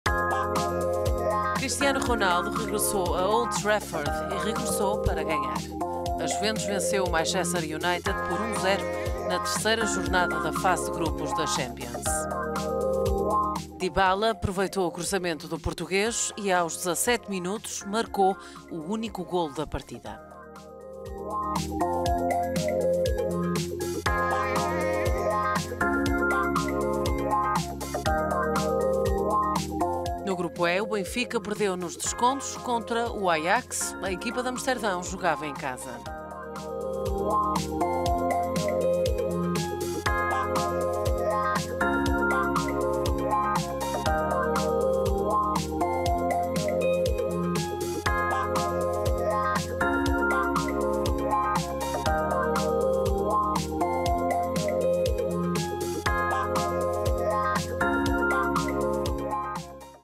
Cristiano Ronaldo regressa a Old Trafford para ganhar
A Juventus venceu o Manchester United por 1-0 com um cruzamento do português…